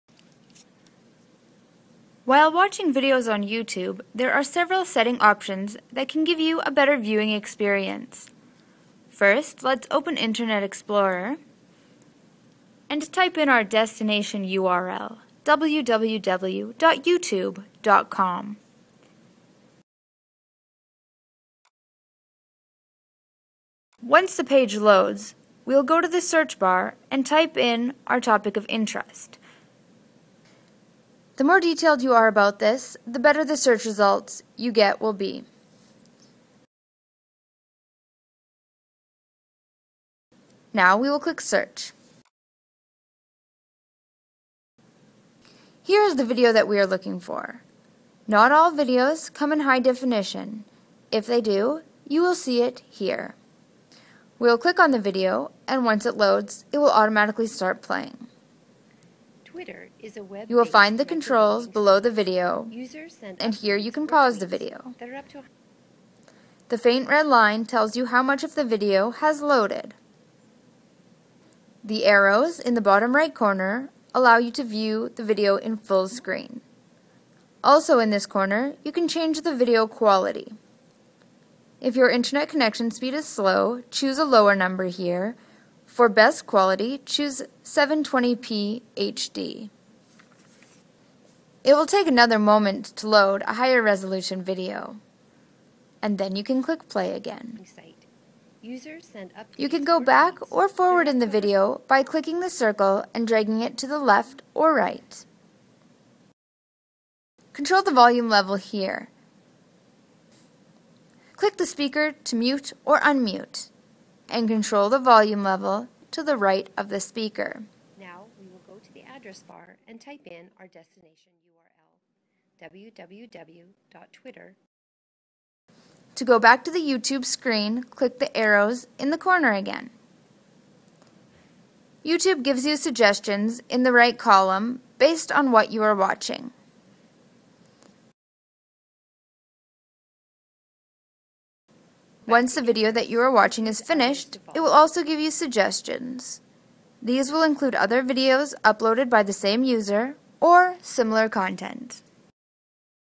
YouTube Video Settings
Learn to adjust settings on YouTube videos (Sound, resolution, etc)